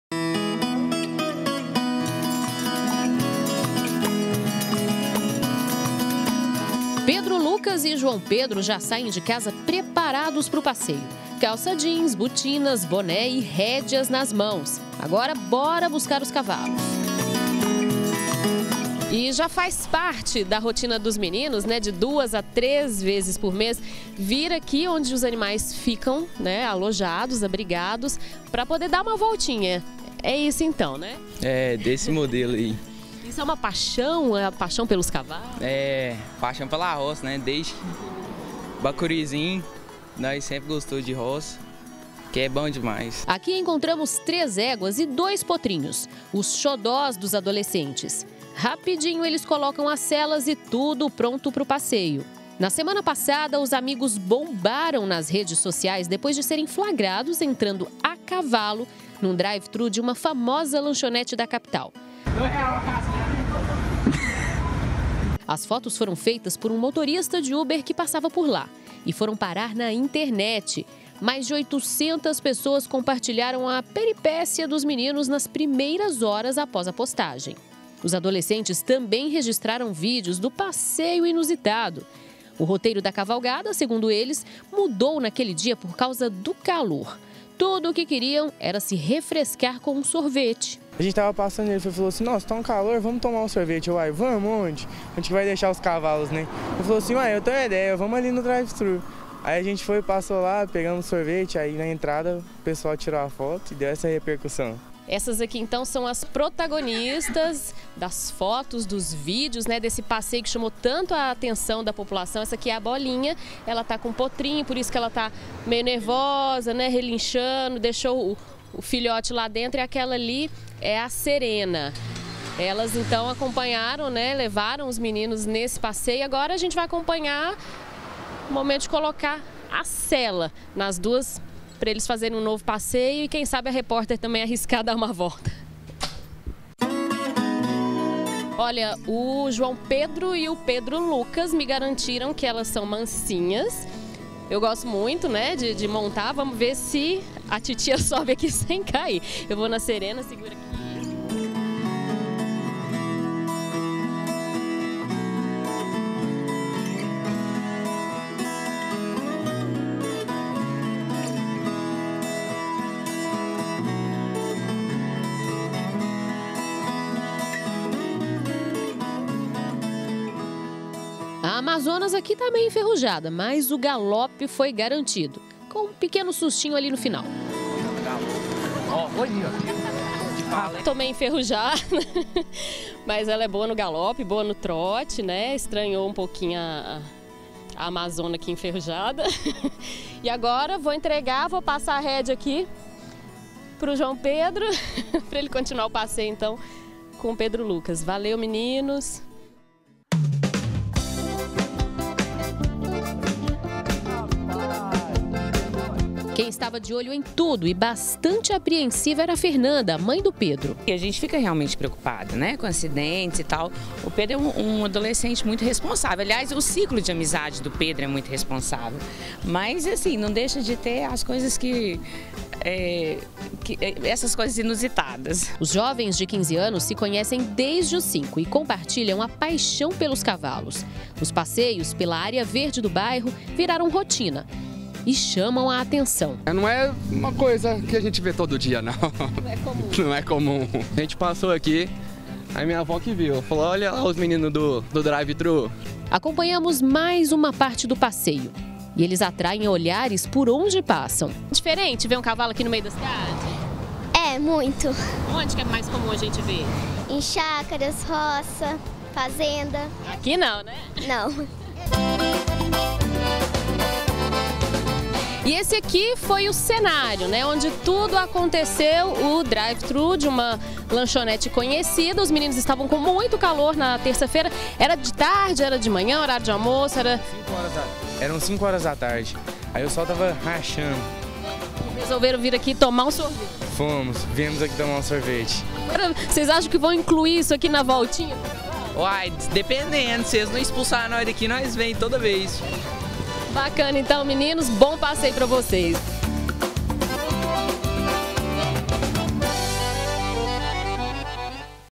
GA - Passeio a cavalo na lanchonete viraliza - 15-01-2018
Você vai conhecer agora dois adolescentes que ficaram famosos depois que foram flagrados entrando no drive thru de uma lanchonete em Goiânia a cavalo.